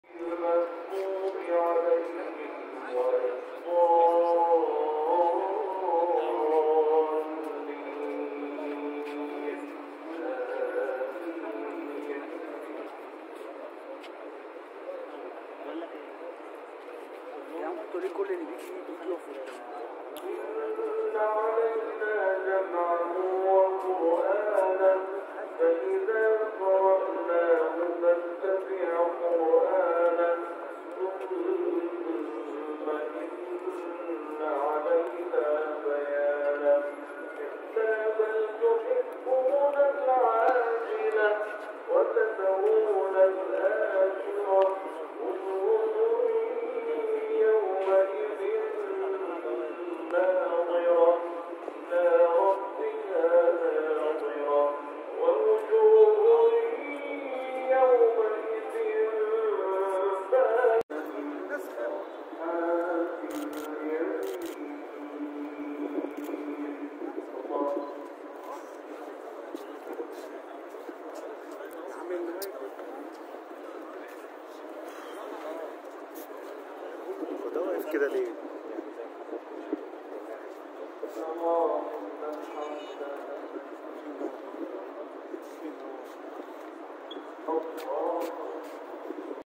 الحزن يخيم على كهربا اثناء حديثه مع صالح جمعة في عزاء أحمد رفعت
الحزن يخيم على كهربا أثتاء حديثه مع صالح جمعة في عزاء أحمد رفعت